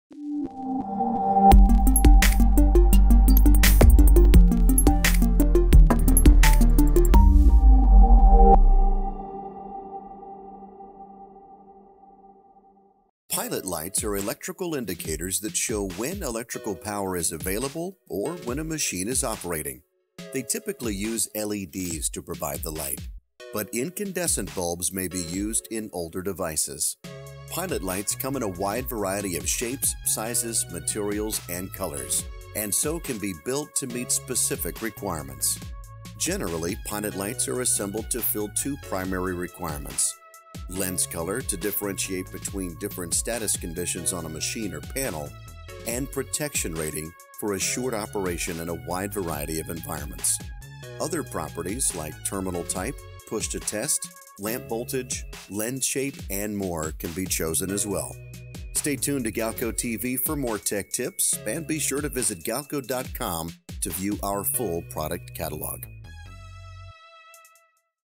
What are Pilot Lights? - A Galco TV Tech Tip
Pilot lights are electrical indicators that show when electrical power is available or when a machine is operating. They typically use LEDs to provide the light, but incandescent bulbs may be used on older devices.

Pilot lights come in wide variety of sizes, shapes, materials and colors, and so can be built to meet specific requirements.